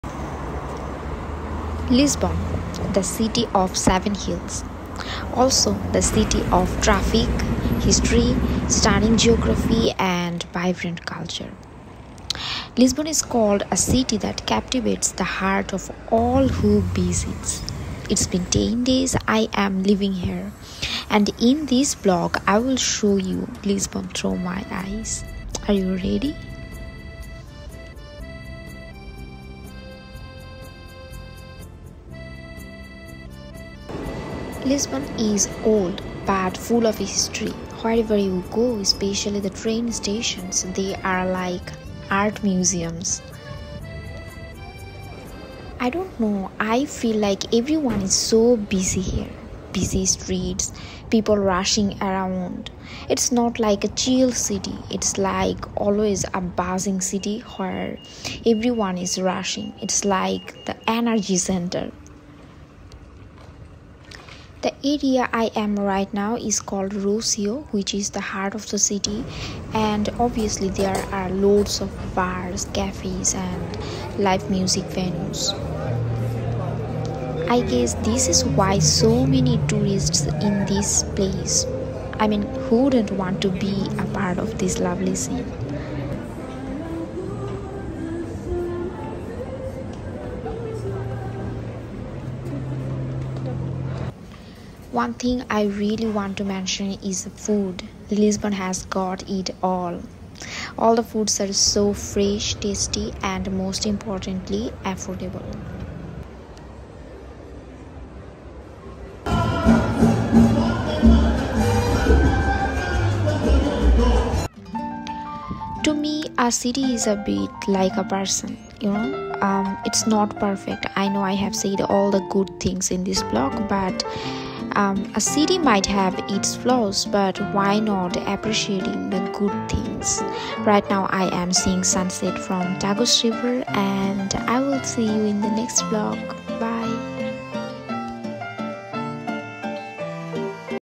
Days in Lisbon🌷
Sharing my experiences in Lisbon.
It is interesting.

The city is vibrant, colorful, and busy but filled with fresh foods. I am especially loving the artwork in every station.